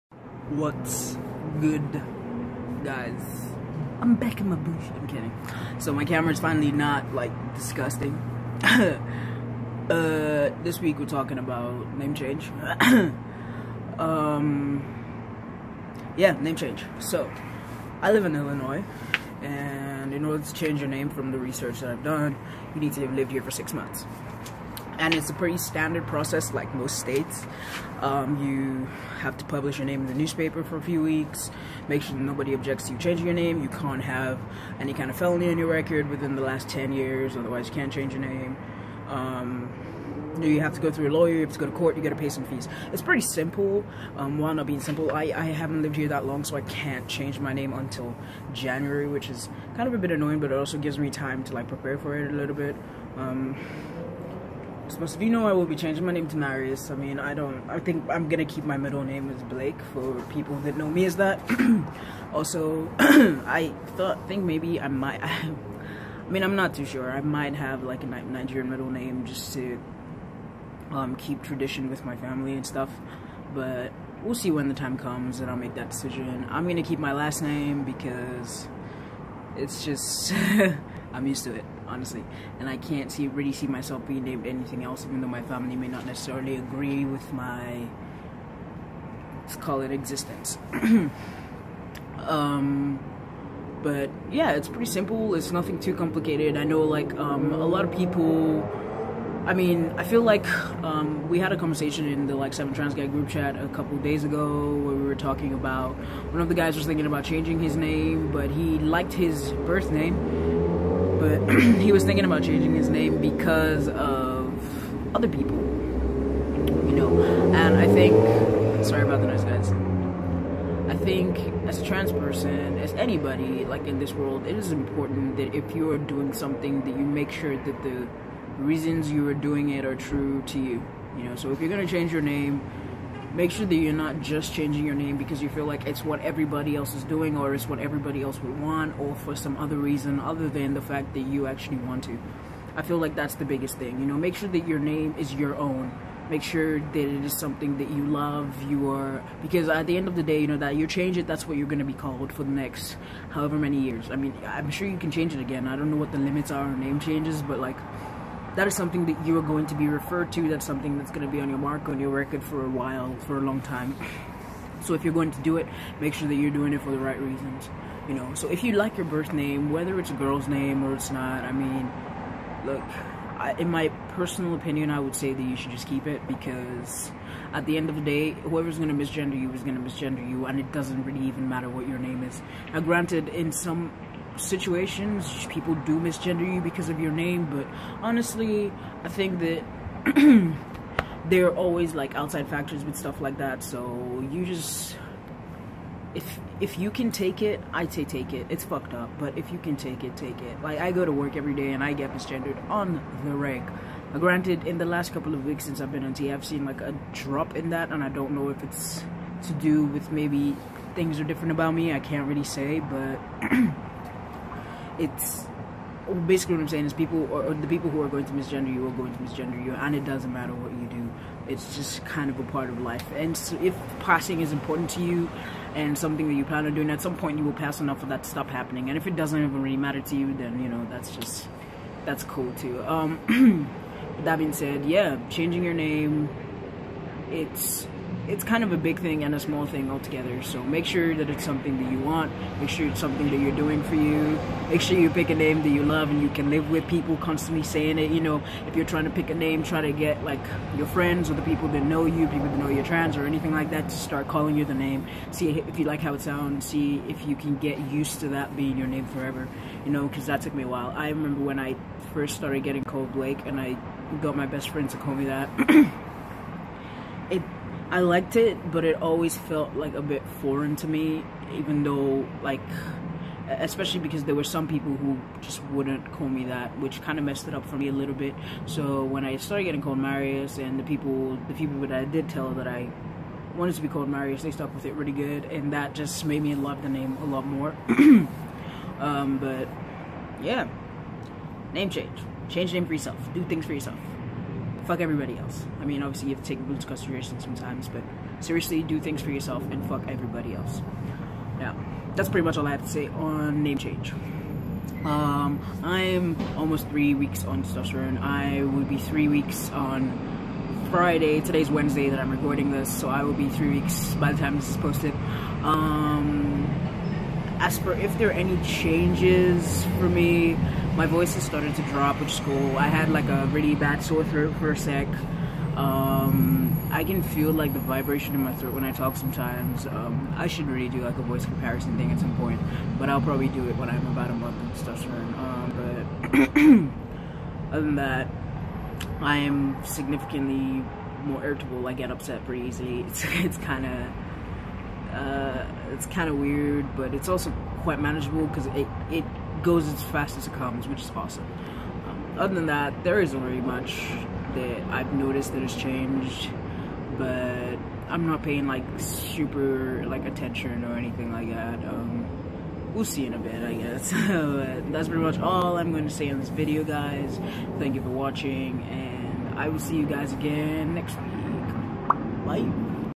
Name Change
My thoughts on changing your name and going about it in Illinois